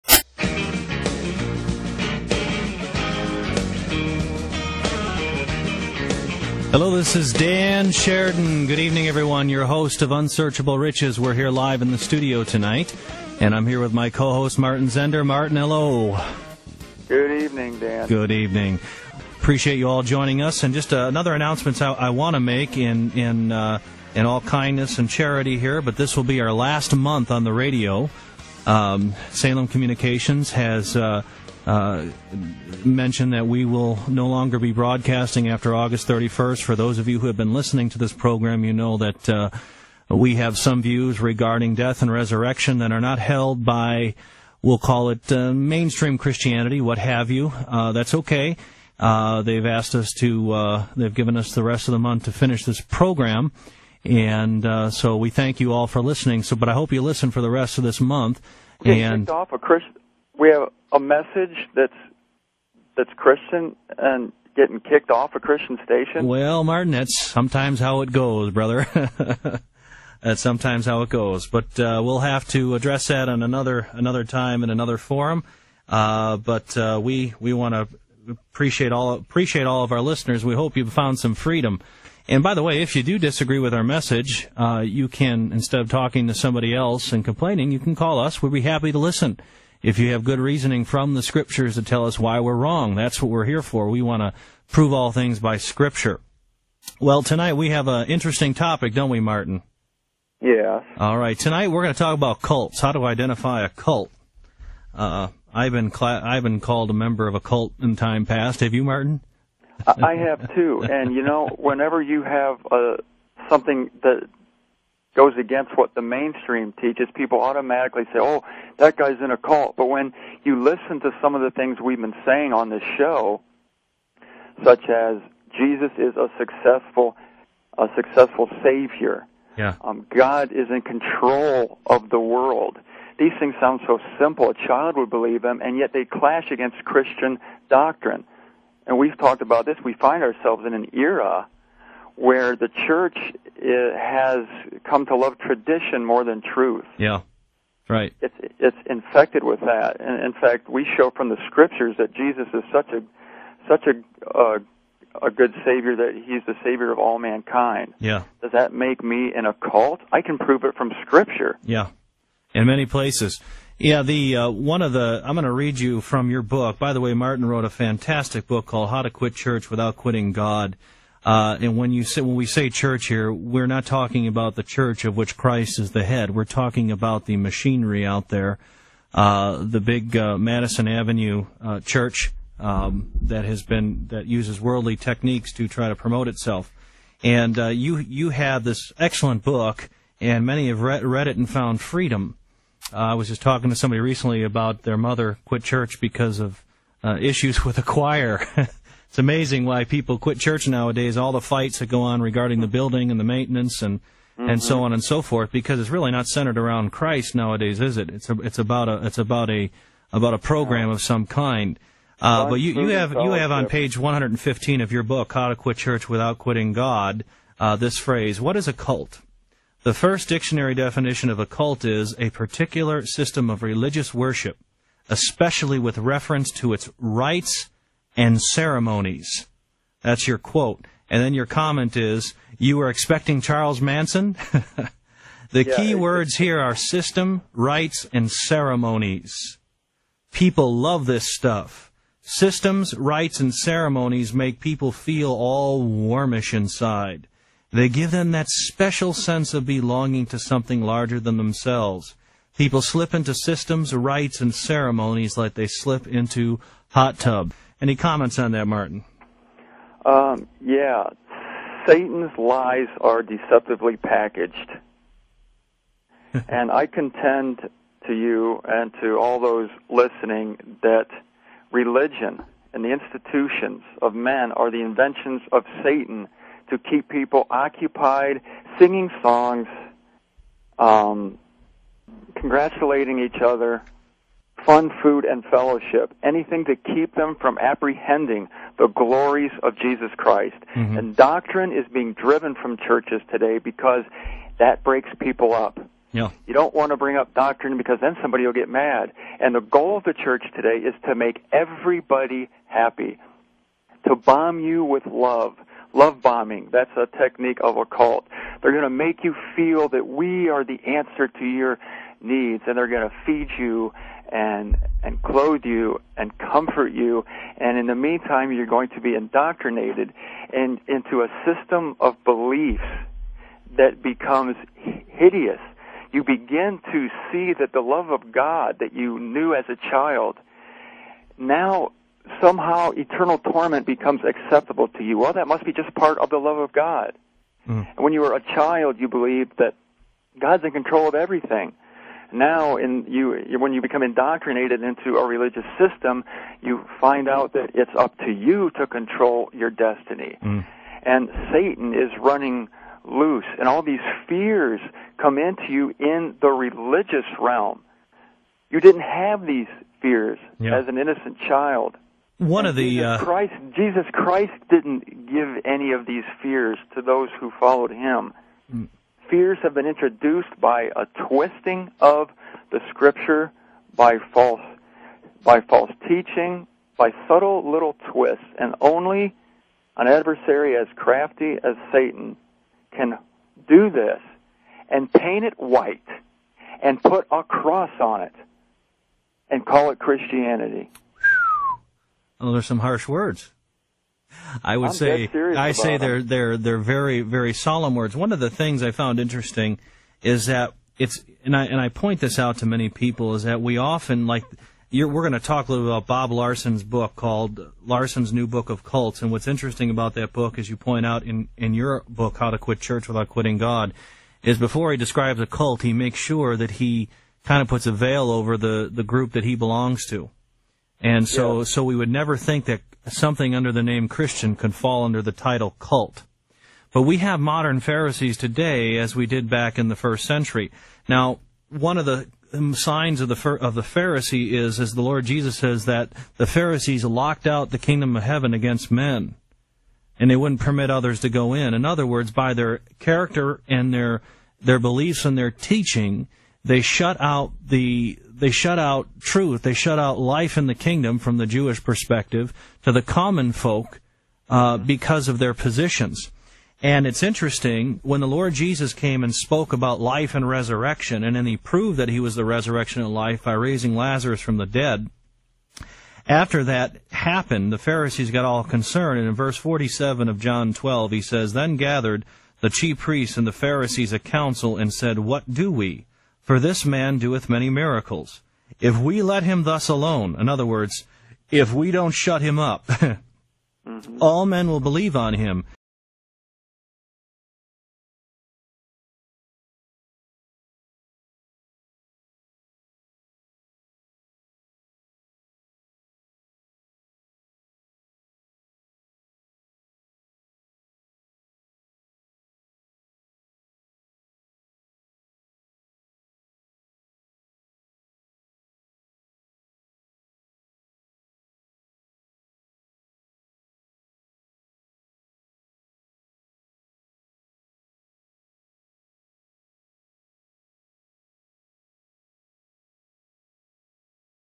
Systems, Rights, and Ceremonies
Dan Sheridan and Martin Zender speak
concerning Cult members .

John ELEVEN , verse 47,48...

The chief priests and the Pharisees ,
then, gathered a Sanhedrin and said ,
" What are we doing, seeing that
this man is doing many signs ?

if we should be leaving him thus ,
ALL will be believing in him ,
and the Romans will come and take away
our place as well as our nation ".